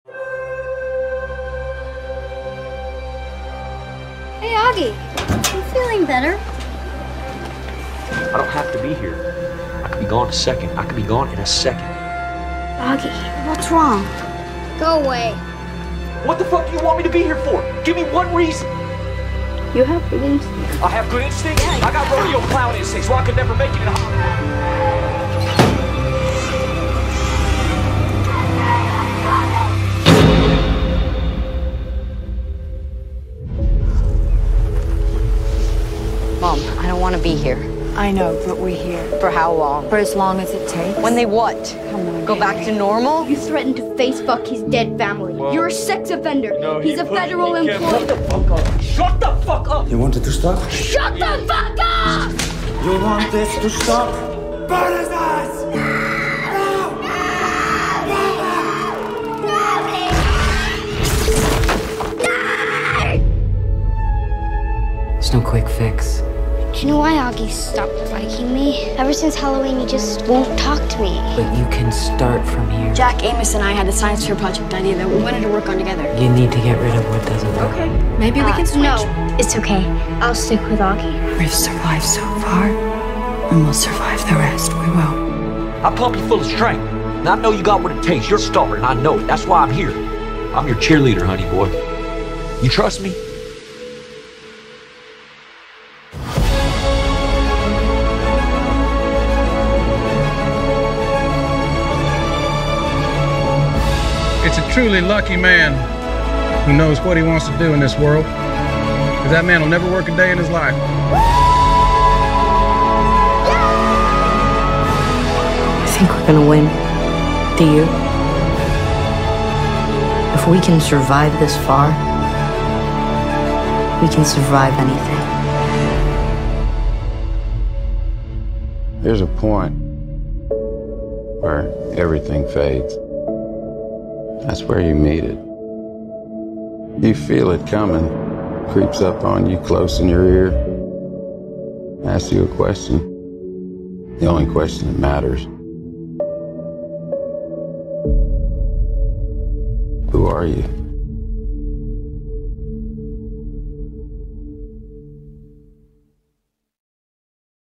Noah Jupe | In Someone Else's Shoes
"If we can survive this far, we can survive anything."

This is a project that I've been working on really hard lately. Noah Jupe's performance in some of the great films in this generation has been truly remarkable and I believe that he deserves a vast recognition. Therefore, I created this short video highlighting some of Noah's notable works during his first five years in the film industry. I hope that you'll enjoy watching it as much as I enjoyed making it.